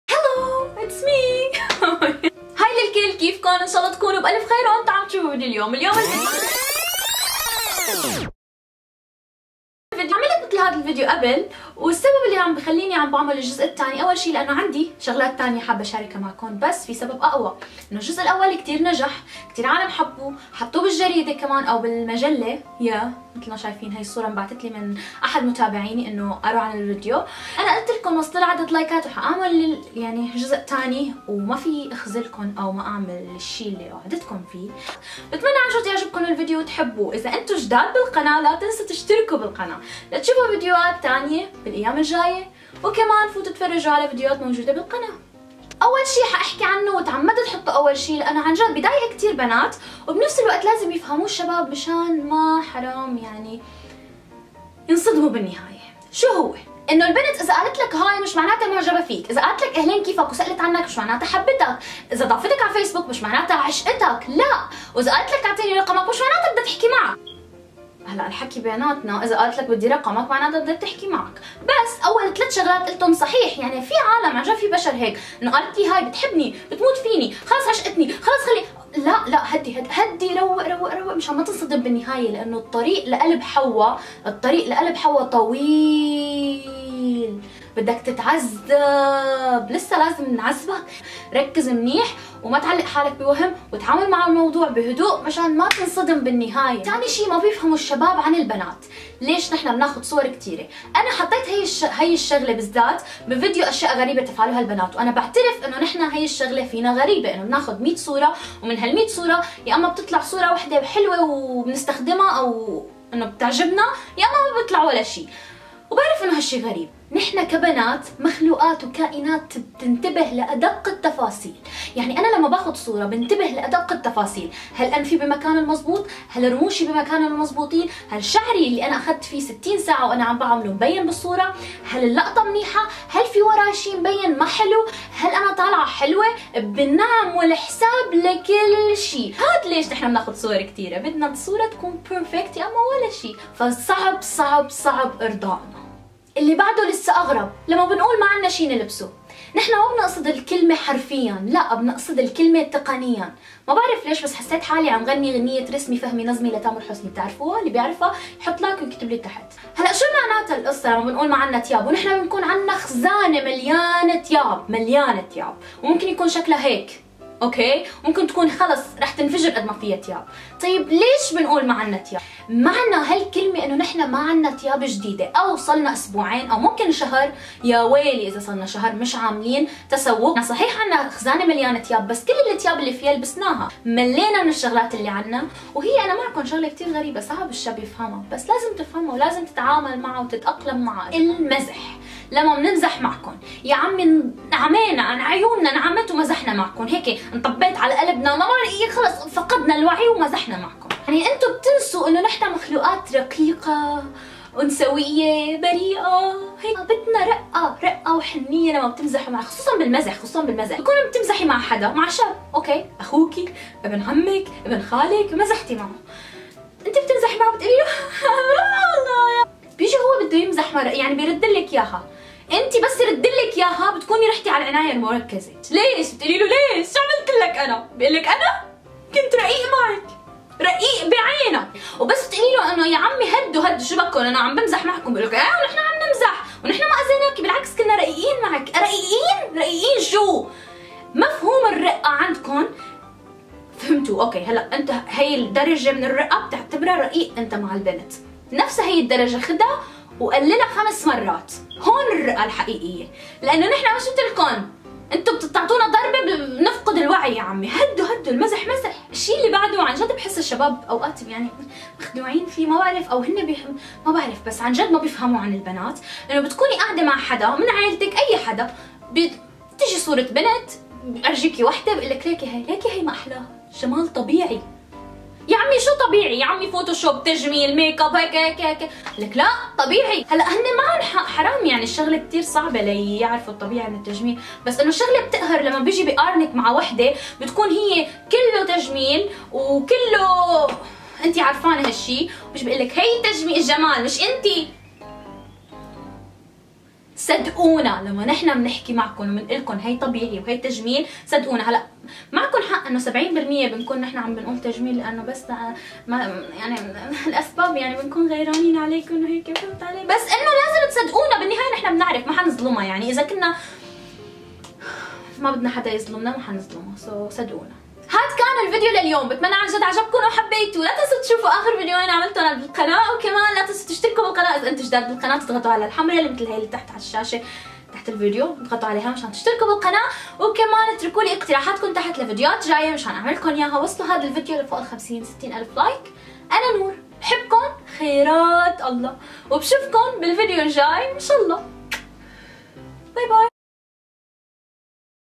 أشياء لا يفهمها الشباب عن البنات !!!
الفيديو اليوم الجزء الثاني ل أشياء لا يفهمها الشباب عن البنات لأنو وعدتكن فية بتمنى يعجبكن وتحبوا لاتنسوا تحطوا لاااايك وشااركوا الفيديو ليشوفوا غيركن

love you more than the ocean xoxoxo